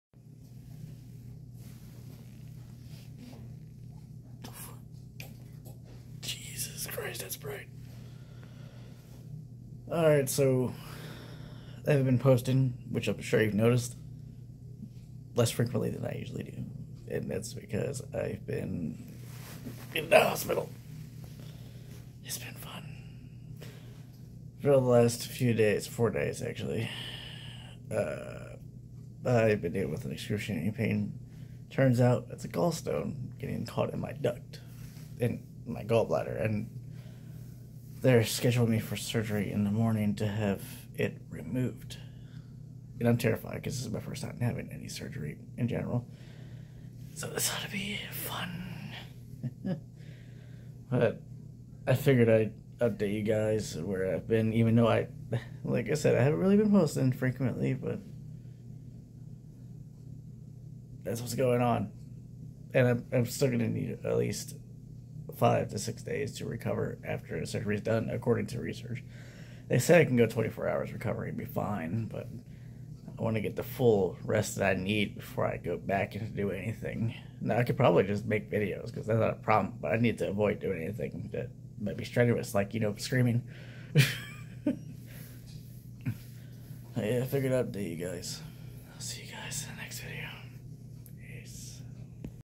Been in the hospital waiting on surgery...
hopefully I'll be out soon.